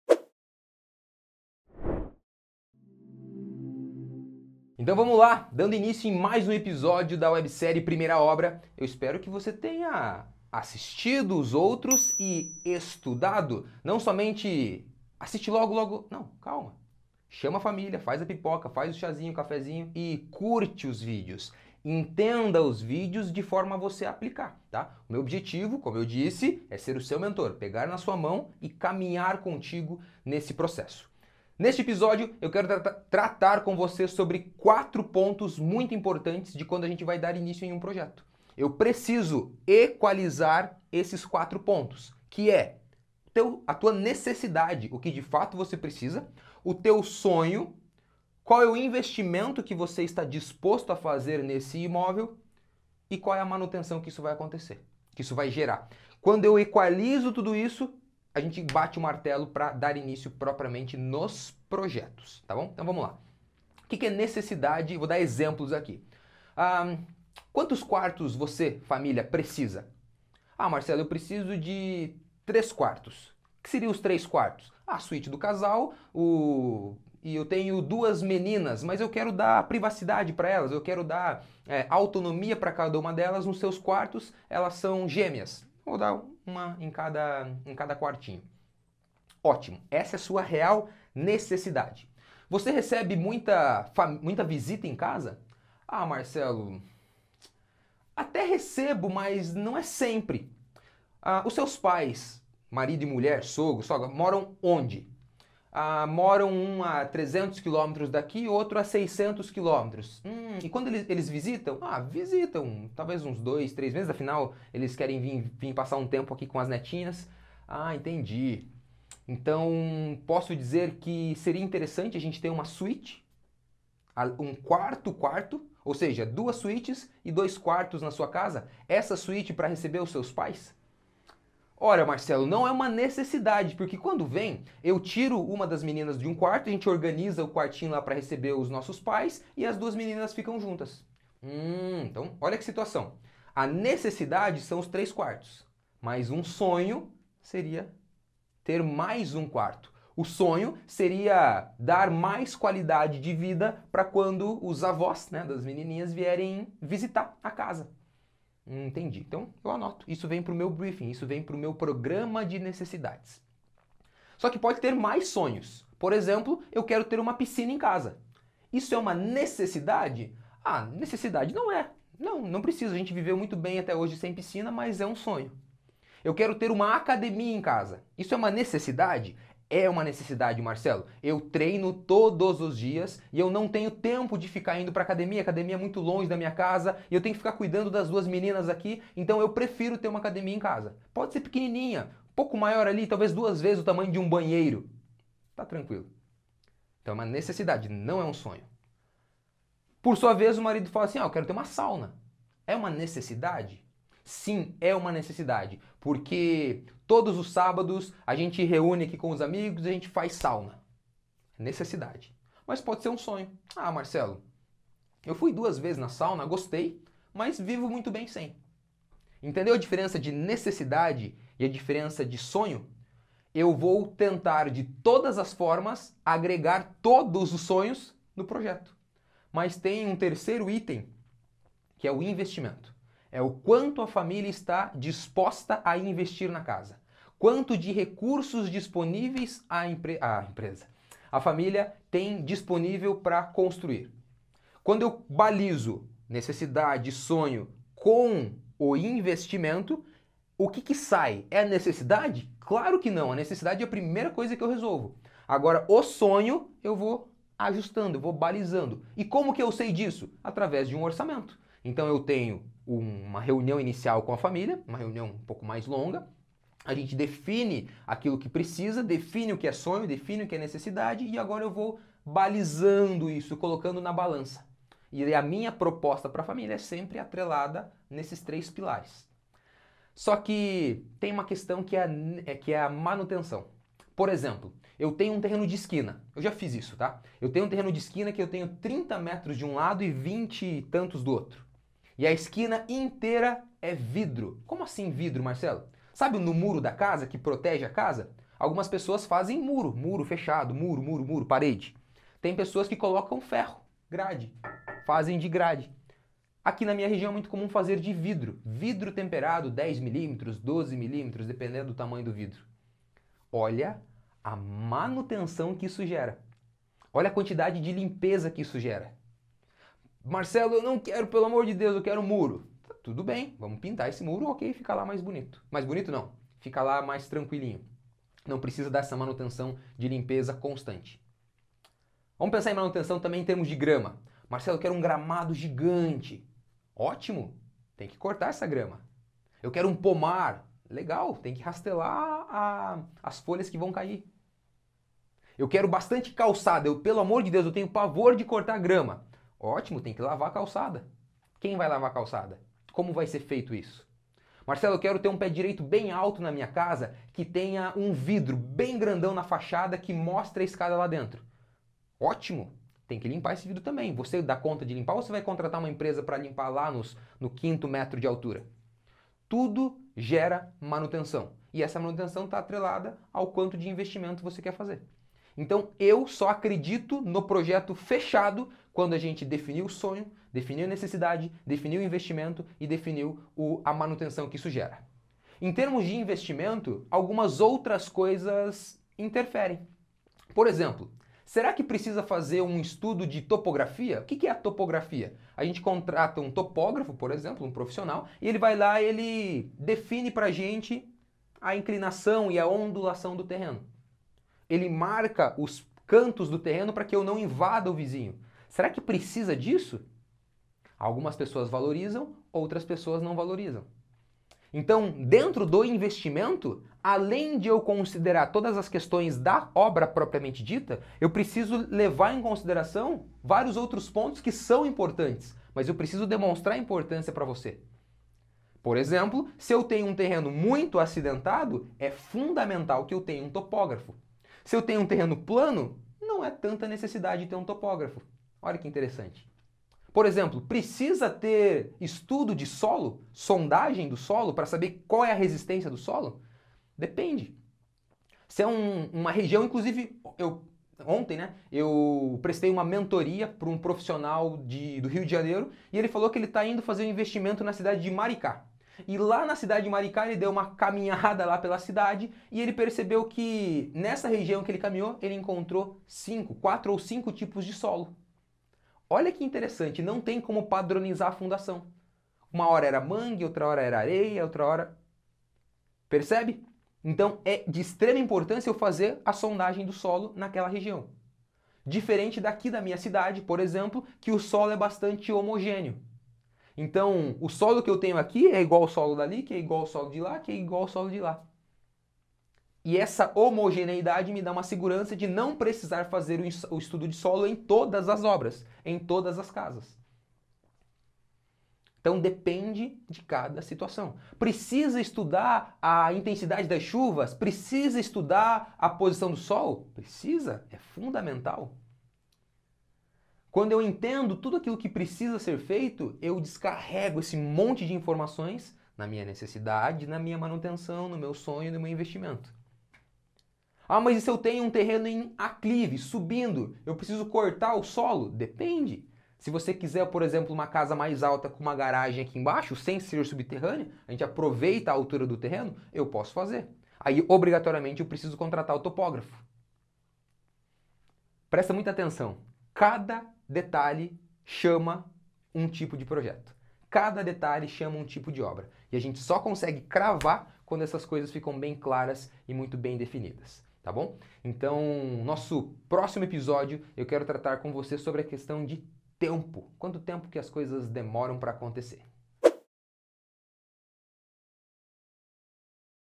PROJETO DE CASA [O Que Considerar] PRIMEIRA OBRA Ep 21
Na hora de fazer o projeto da casa, cada profissional leva em consideração aquilo que ele acha importante.

Aqui na Akira Design Residencial, nós acreditamos que para termos um projeto residencial bem feito, deve-se equalizar a Real Necessidade que a família tem, os Sonhos, a Manutenção que essa casa irá gerar ao longo do tempo e quanto a família está disposta a Investir na construção da casa.

Essas informação vão sendo levantadas durante o processo criativo do projeto arquitetônico.

Não importa se é um projeto de alto padrão, uma casa moderna, um sobrado simples e pequeno, nós levamos em consideração os quatro pontos.

SEJA MEMBRO DO CANAL E APOIE AS FAMÍLIAS SEM CONDIÇÕES!!!